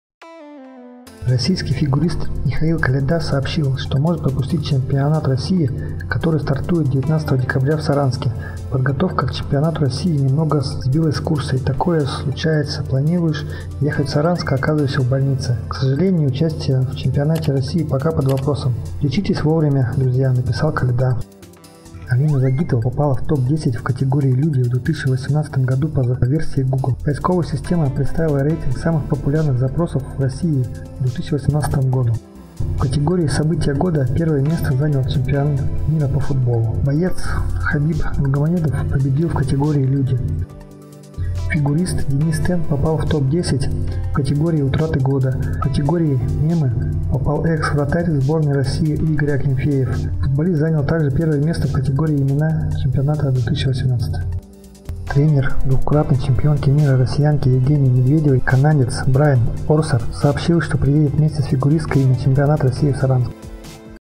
В топе 10 по версии Google Загитова, Хабиб и Акинфеев – Спортсмены года
Российский фигурист Михаил Коляда сообщил, что может пропустить чемпионат России, который стартует 19 декабря в Саранске.

«Подготовка к чемпионату России немного сбилась с курса… И такое случается. Планируешь ехать в Саранск, а оказываешься в больнице.

К сожалению, участие в ЧР пока под вопросом. Лечитесь вовремя, друзья!» – написал Коляда.

Алина Загитова попала в топ-10 в категории «Люди» в 2018 году по версии Google

Поисковая система представила рейтинги самых популярных запросов в России в 2018 году.

В категории «События года» первое место занял чемпионат мира по футболу.

Боец UFC Хабиб Нурмагомедов победил в категории «Люди».

Фигурист Денис Тен попал в топ-10 (9-е место) в категории «Утраты года».

В категорию «Мемы» попал экс-вратарь сборной России Игорь Акинфеев (8-е место). Футболист занял также первое место в категории «Имена ЧМ-2018».

«События года»:

1. Чемпионат мира по футболу  2. Пхенчхан-2018  3. Кемерово  4. Бой Хабиба и Макгрегора  5. Выборы президента России  6. Керчь  7. Чемпионат мира по хоккею  8. Ан-148  9. Свадьба принца Гарри  10. Затмение 27 июля

«Имена ЧМ-2018»:

1. Игорь Акинфеев  2. Артем Дзюба  3. Александр Головин  4. Марио Фернандес  5. Станислав Черчесов  6. Федор Смолов  7. Денис Черышев  8. Лука Модрич  9. Килиан Мбаппе  10. Сергей Игнашевич

«Люди»:

1. Хабиб Нурмагомедов  2. Меган Маркл  3. Настя Рыбка  4. Павел Грудинин  5. Президент Хорватии  6. Александр Кокорин  7. Алина Загитова  8. Илон Маск  9. Конор МакГрегор  10. Монеточка

Орсер вместе с Медведевой приедет в Саранск на чемпионат России. Тренер двукратной чемпионки мира россиянки Евгении Медведевой канадец Брайан Орсер сообщил , что приедет вместе с фигуристкой на чемпионат России в Саранск.

Канал World Events  посвященный  событиям в мире,